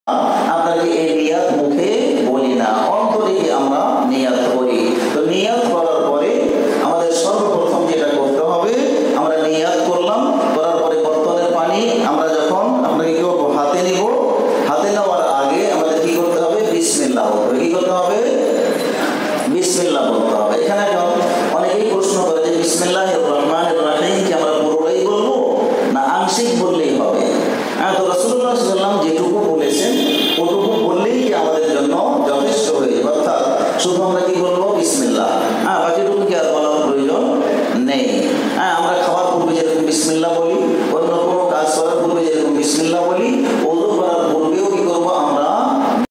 অজুর শুরুতে বিসমিল্লাহ পুরোটা বলতে হয় নাকি শুধু বিসমিল্লাহ.শাইখ আবু হানিফ মাদানী
শাইখ আবু হানিফ মাদানী
banglawaz 2018